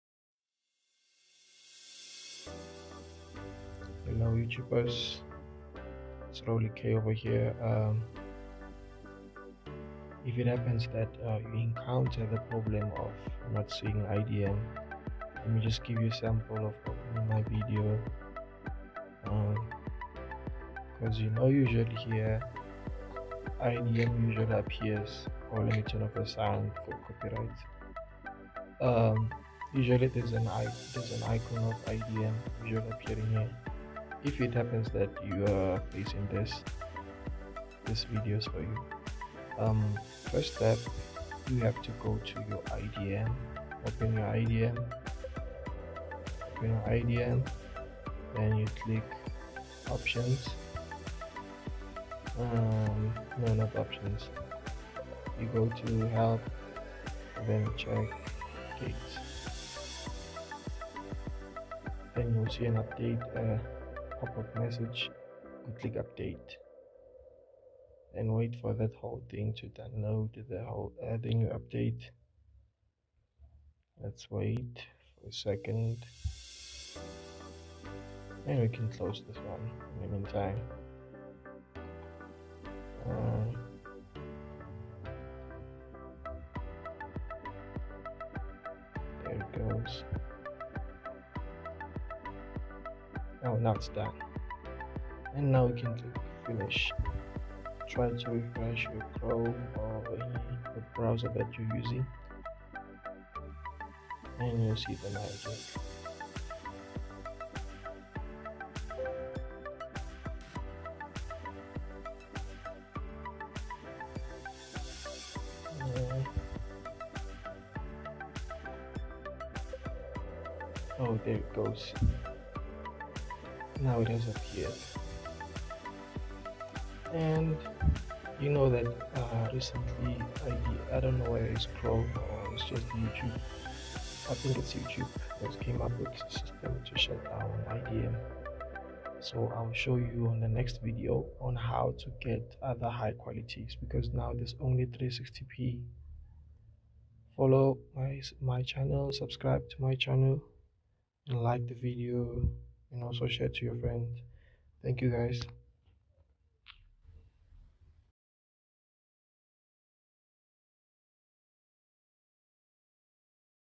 How to Fix internet download Manager (IDM) Download Button Missing on YouTube (100% guaranteed✅)
Is your Internet Download Manager (IDM) download button not showing on YouTube in Chrome or Firefox? Don't worry—this video walks you through quick and effective fixes to get the IDM button back and working on both browsers.

✅ Works for:

1. Google Chrome

2. Mozilla Firefox

3. Microsoft Edge